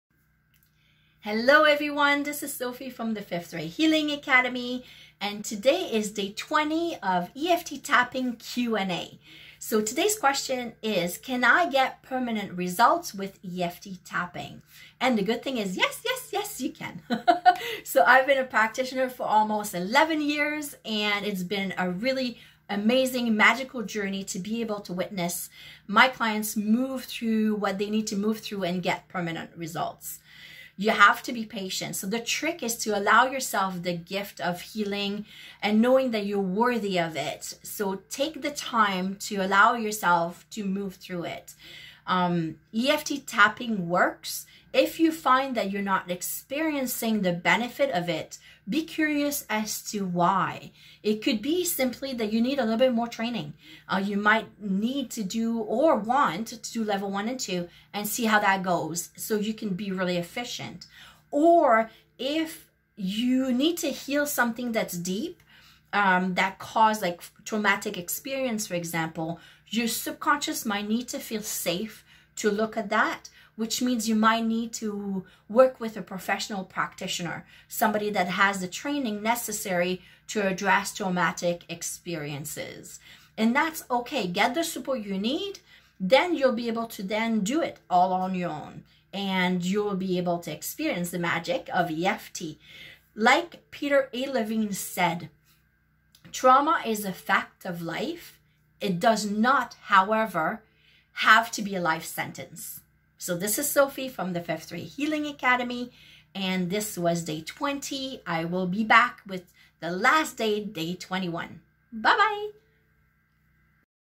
Can I get permanent results with EFT/tapping?
It’s Day 20 of our 21 Days of EFT/Tapping Q&As. As Sophie says “Yes! Yes! Yes!” In today’s short video, Sophie explains one way to set yourself up for success and a couple of reasons why the benefits of tapping can sometimes feel out of reach.

Check out the 5th Ray Healing Academy's EFT/Tapping classes and programs